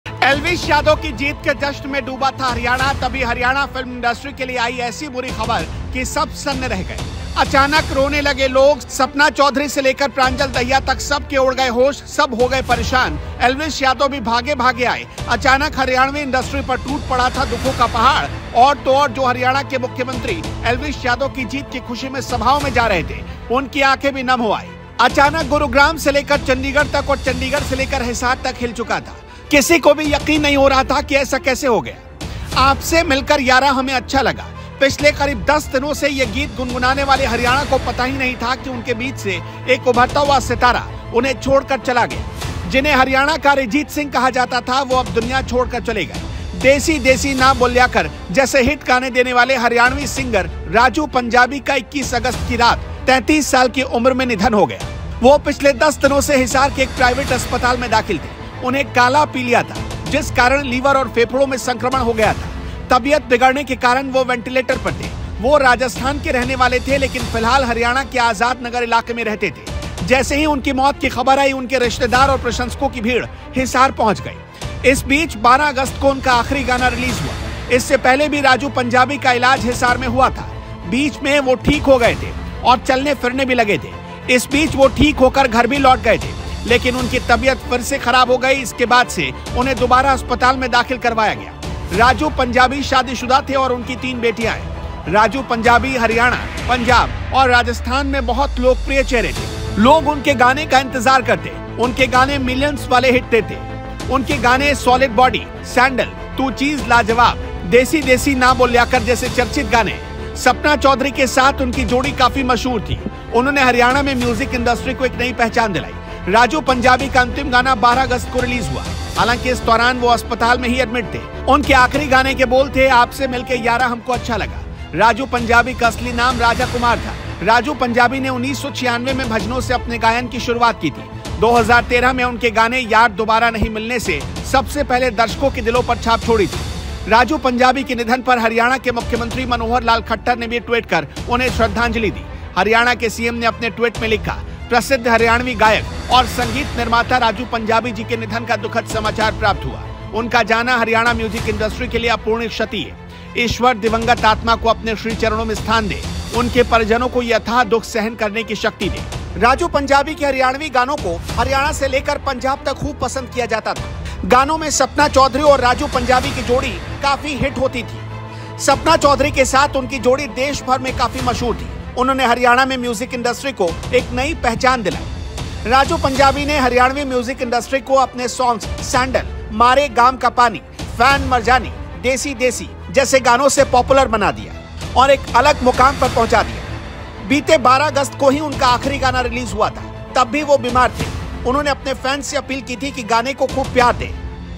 Sapna Chaudhary पर क्यों टूटा दुखों का पहाड़? Elvish Yadav की आंखें क्यों भर आईं?RajuPunjabi
Sapna Chaudhary पर क्यों टूटा दुखों का पहाड़? Elvish Yadav की आंखें क्यों भर आईं?